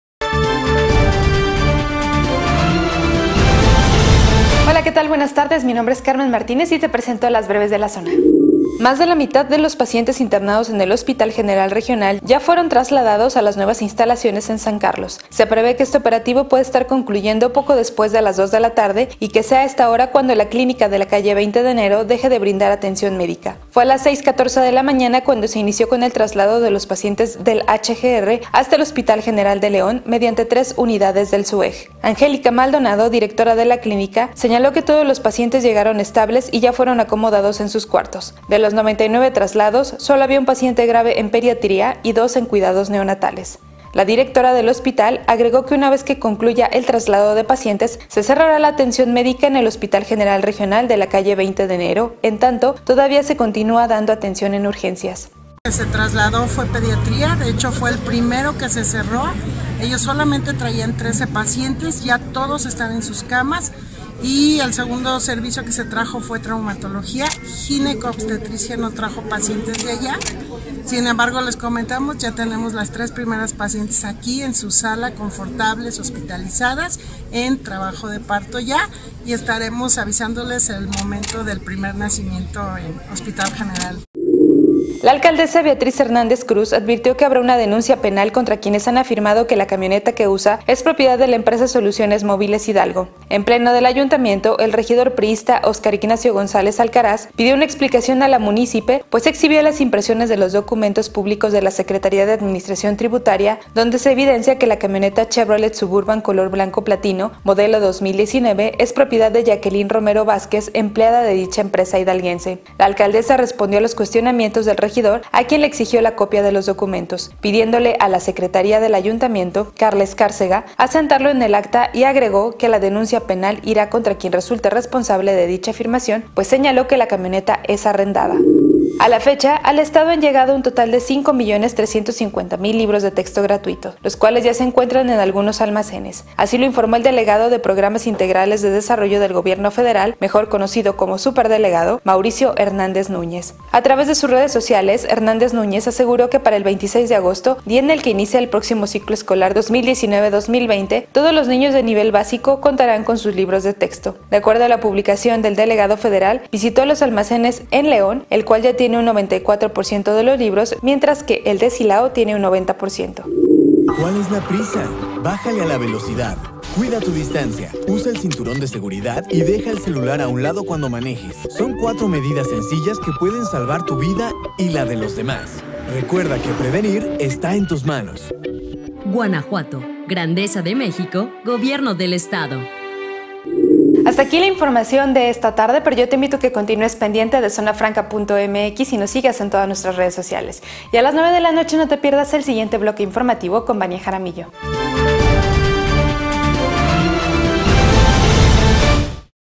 Llegan pacientes al nuevo Hospital General de León | Breves de la Zona
Hoy en las BrevesDeLaZona📰💻 con Carmen Martínez
- Llegan pacientes al nuevo Hospital General de León; el antiguo HGR, a punto de cerrar sus puertas.
- Anuncia Alcaldesa de Salamanca denuncias penales: “camioneta es arrendada”, defiende.
- Gobierno federal asegura que llegará totalidad de libros de texto Guanajuato.